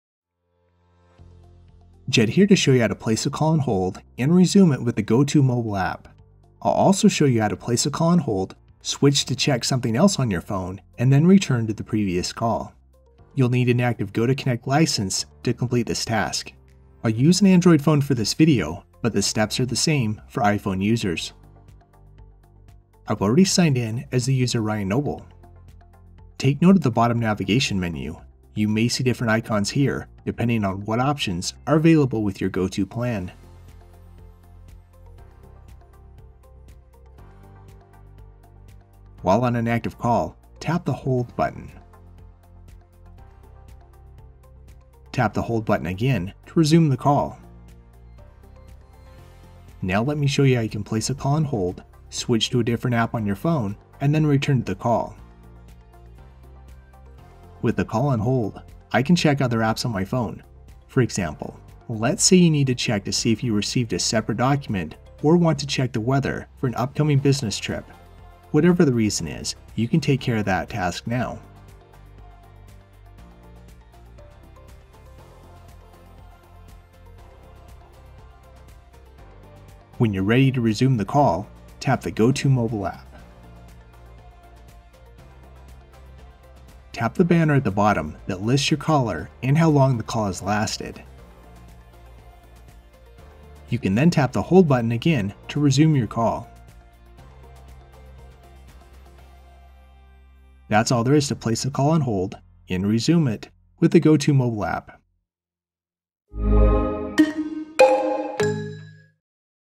Place a Call on Hold with the GoTo Mobile App
This video demonstrates placing a call on hold with the GoTo Mobile app. You can also place a call on hold, check something else on your phone, and then return to the call.

0:00 Introduction
0:35 Place a call on hold and resume the call
0:49 Check another phone app while a call is on hold
1:39 End credits